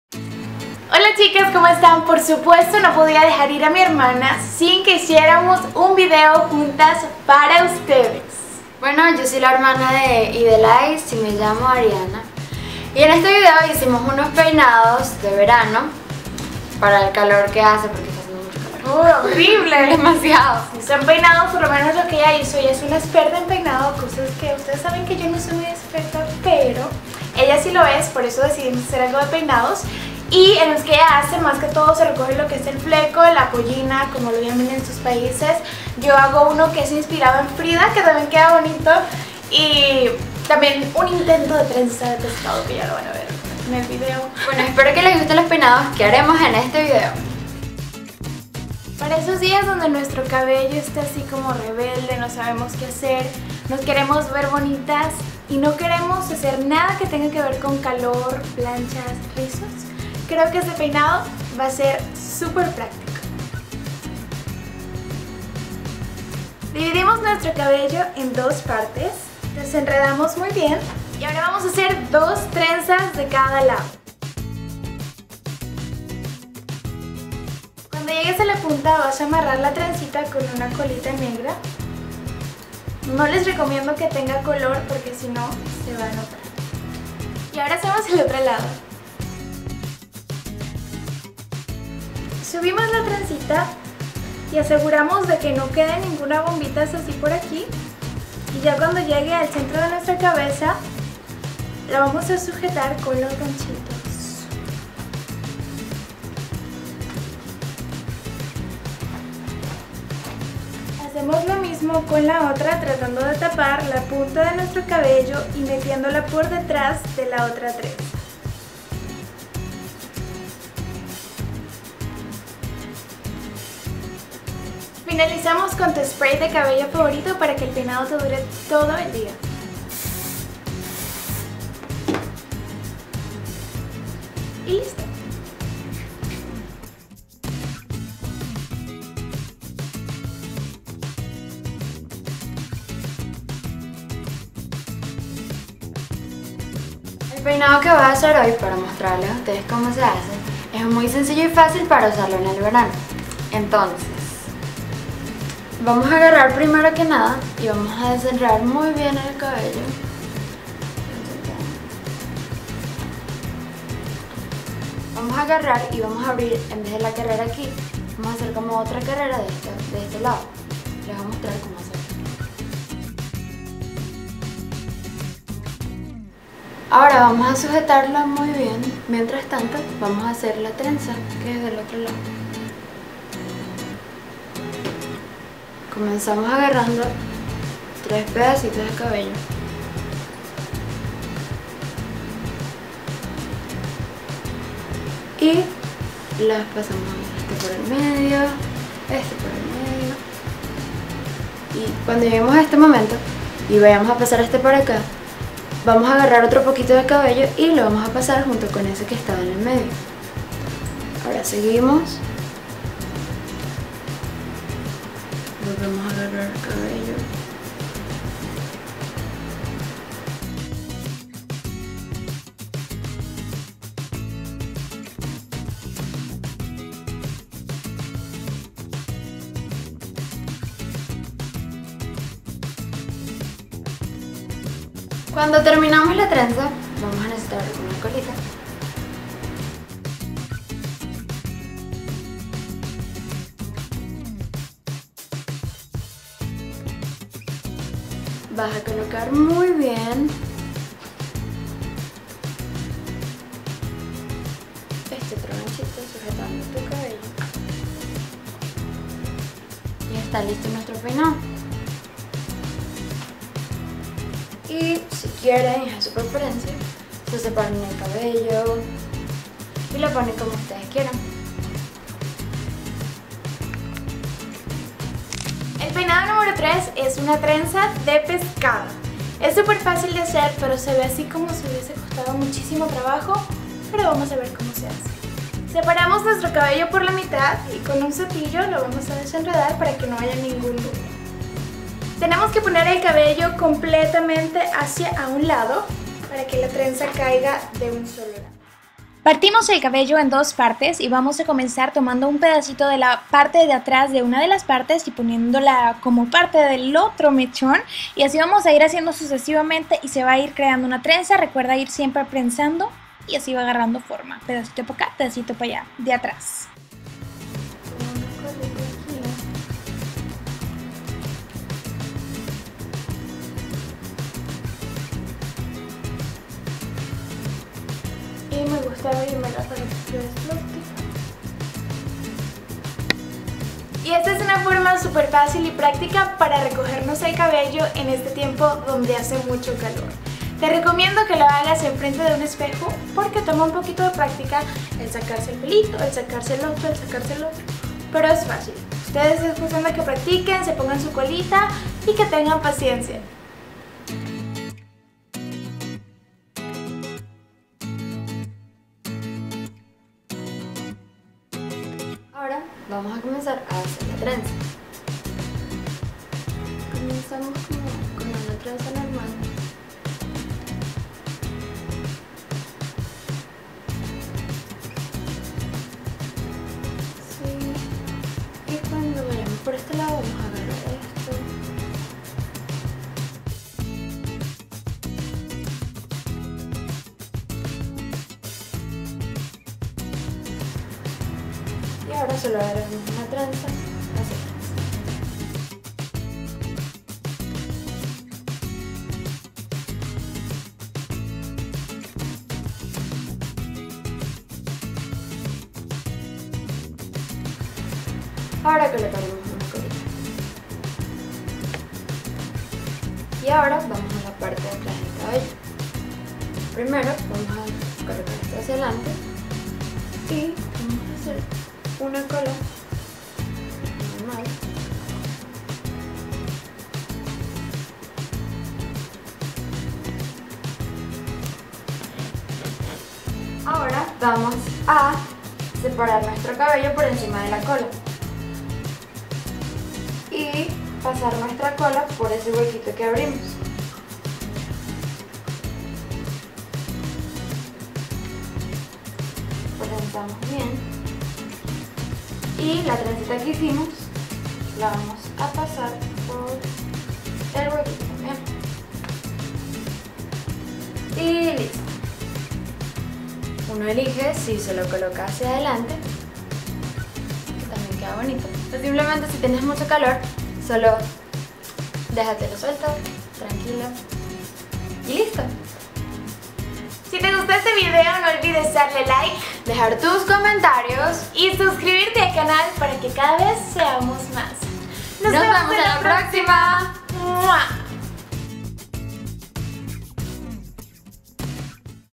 4 Peinados Para El Calor/Escuela Faciles y Bonitos - Ydelays
En este video mi hermana y yo les vamos a enseñar 4 peinados diferentes que pueden usar para ir a la escuela o simplemente cuando quieran verse súper coquetas en esta época de calor!!! Espero que disfruten de este video chicas lindas!
LAS ADORO con todo mi corazón y mi hermana y yo le mandamos un abrazo GIGANTEEEE!